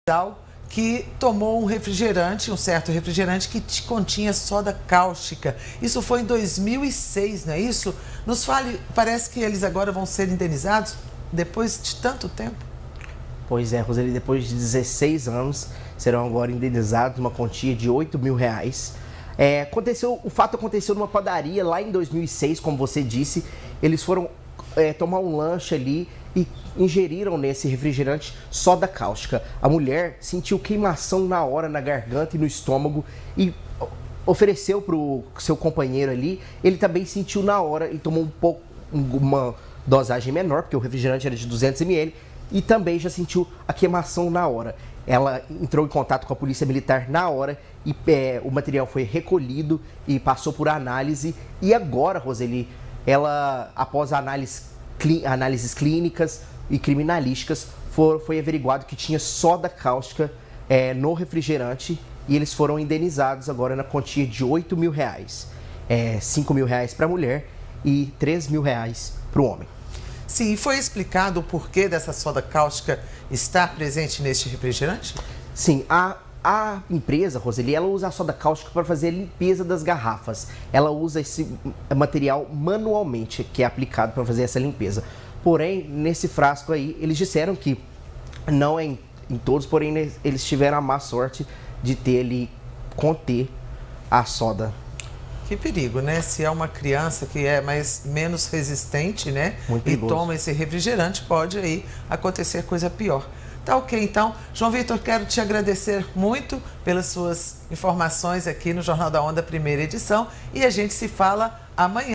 CASAL DE DIVINÓPOLIS QUE TOMOU REFRIGERANTE COM SODA CÁUSTICA SERÁ INDENIZADO PELA MARCA
| REGIÃO |

UM CASAL DE DIVINÓPOLIS APRESENTOU EPISÓDIOS DE QUEIMAÇÃO E ARDÊNCIA APÓS TOMAR UM REFRIGERANTE EM UMA PADARIA NO ANO DE 2006, DEPOIS DE 16 ANOS, OS TESTES CONFIRMARAM QUE HAVIA SODA CÁUSTICA NA BEBIDA, E O CASAL SERÁ INDENIZADO EM R$ 8 MIL.